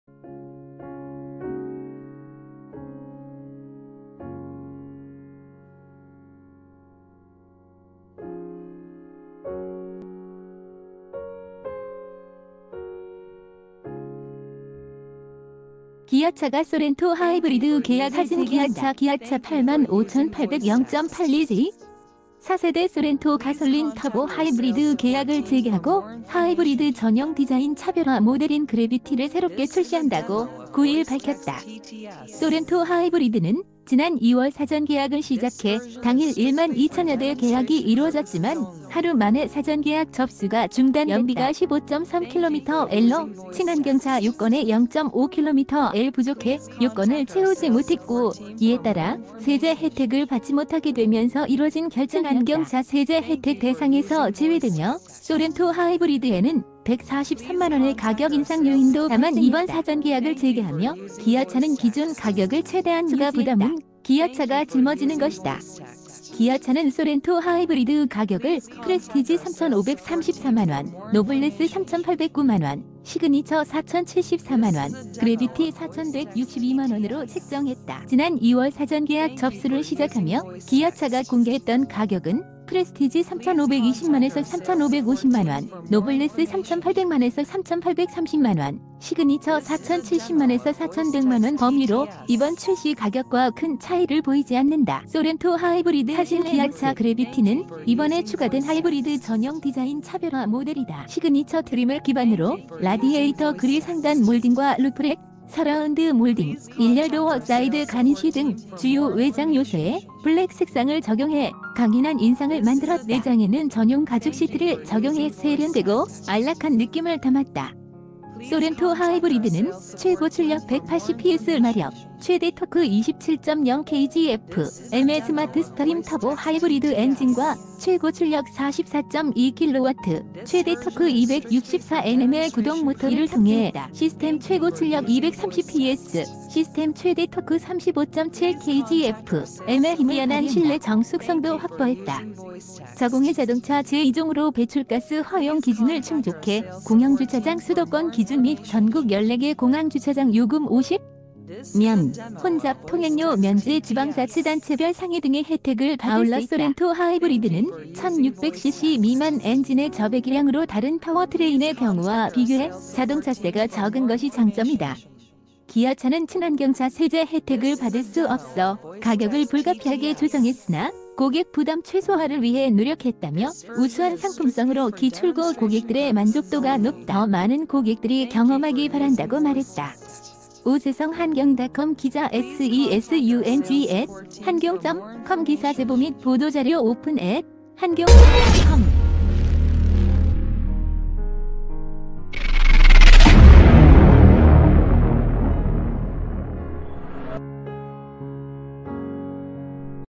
돌아온 쏘렌토 하이브리드…저공해車 가격 '그대로'
돌아온 쏘렌토 하이브리드…저공해車 가격 '그대로' 기아차가 쏘렌토 하이브리드 계약을 재개한다. 사진=기아차 기아차(85,800 +0.82%)가 4세대 쏘렌토 가솔린 터보 하이브리드 계약을 재개하고 하이브리드 전용 디자인 차별화 모델인 ‘그래비티’를 새롭게 출